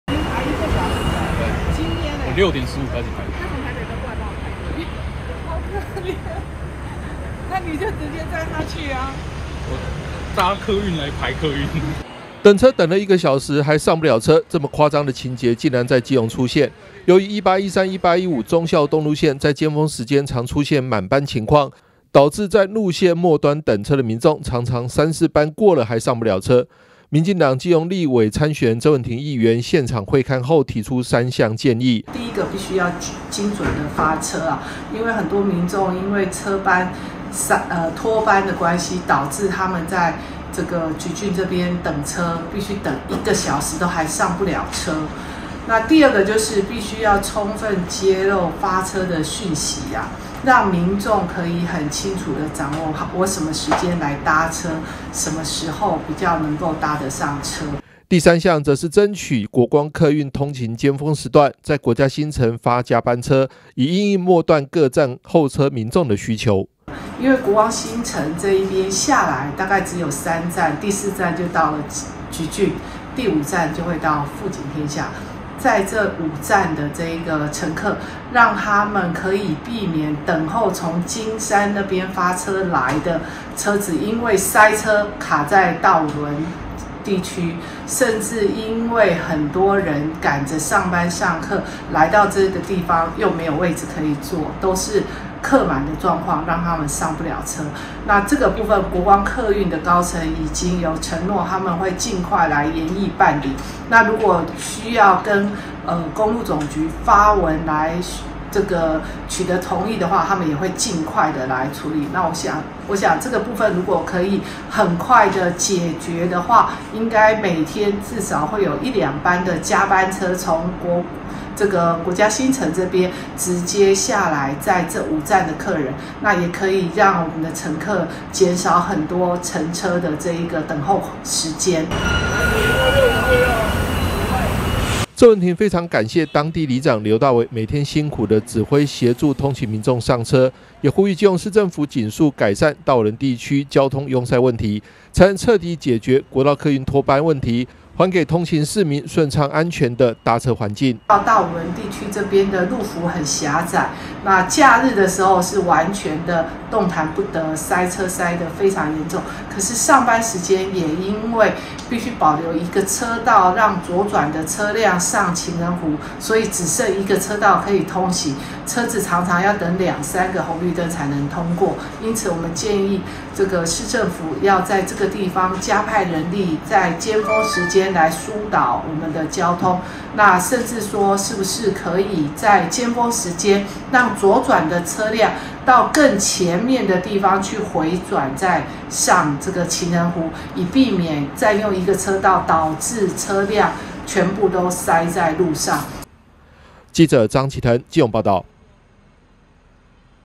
脫班 滿班|國道 客運|通勤 交通|鄭文婷 基隆|發車 資訊|2023 中嘉新聞|搭客運要等一小時! 1813、1815路線常滿班 鄭文婷爭取發加班車
搭客運竟要等一個小時 基隆通勤民眾等到懷疑人生 鄭文婷會勘提解方

基隆國道客運脫班、滿班問題頻傳，其中1813、1815忠孝東路路線等車民眾甚至排了一個小時還上不了車，民進黨基隆立法委員參選人鄭文婷一早特別到候車亭現場了解情況，鄭文婷表示已經與國光客運方面溝通，除了要求精準發車及充分揭露發車資訊外，更希望爭取在國家新城發加班車因應通勤民眾需求，鄭文婷也呼籲基隆市政府儘速改善大武崙交通壅塞問題，還給市民一個順暢安全的搭車環境。

等車等了一個小時上不了車，這麼誇張的情節竟然在基隆出現，由於1813、1815忠孝東路線在尖峰時間常出現滿班情況，導致在路線末段等車的民眾常常三四班過了還上不了車，民進黨基隆立委參選人鄭文婷議員現場會勘後提出三項建議，首先是要求精準發車及充分揭露發車訊息。

第三項則是爭取國光客運通勤尖峰時段在國家新城發加班車，以因應末段各站候車民眾的需求。

鄭文婷非常感謝當地里長劉大維每天辛苦的指揮協助通勤民眾上車，也呼籲基隆市政府儘速改善大武崙地區交通雍塞問題，才能徹底解決國道客運脫班問題，還給通勤市民順暢安全的搭車環境。